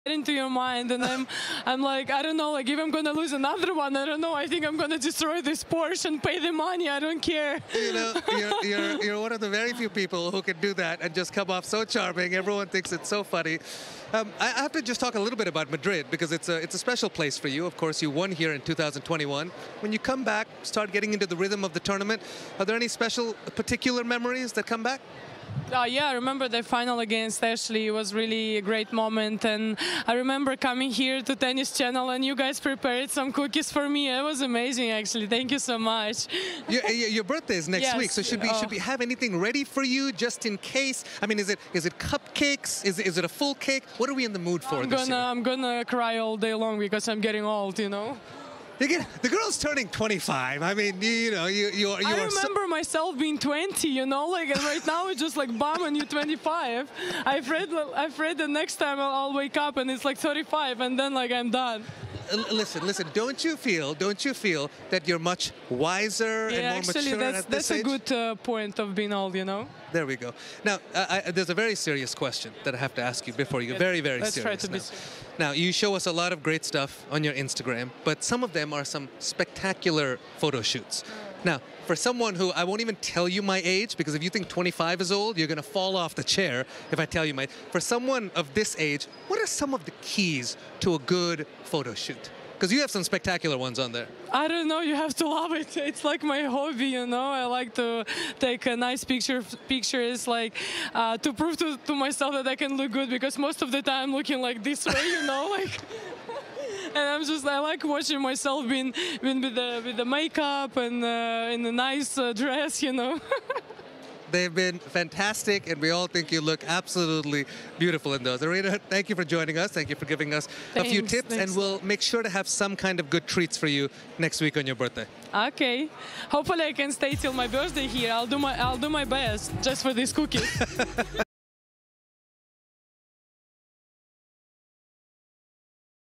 Aryna Sabalenka talks about her 2021 Madrid Title | 2023 Madrid Third Round
Aryna Sabalenka sits down with Tennis Channel